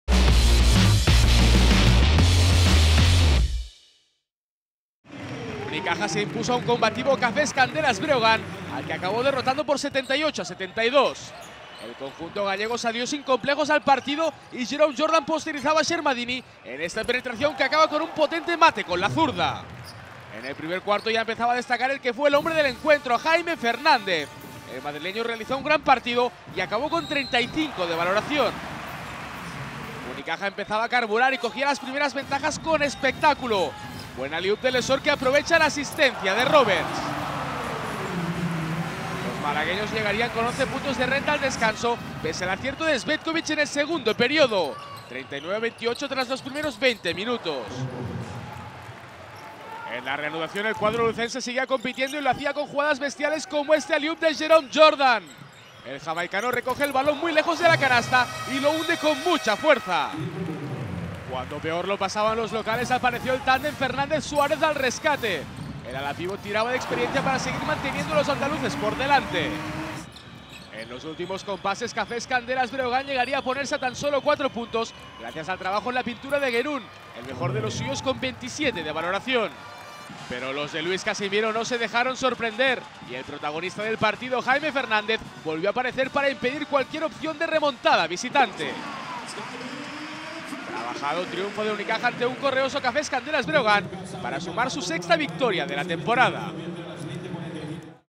Unicaja - Cafés Candelas Breogán (78-72) RESUMEN // Jornada 8 Liga Endesa
Unicaja logró una trabajadísima victoria ante un Cafés Candelas Breogán que no se dio nunca por vencido. El partidazo de Jaime Fernandez llevó en volandas a un Unicaja que a punto estuvo de verse sorprendido en el tramo final por una reacción de los gallegos comandada por Cvetkovic y Gerun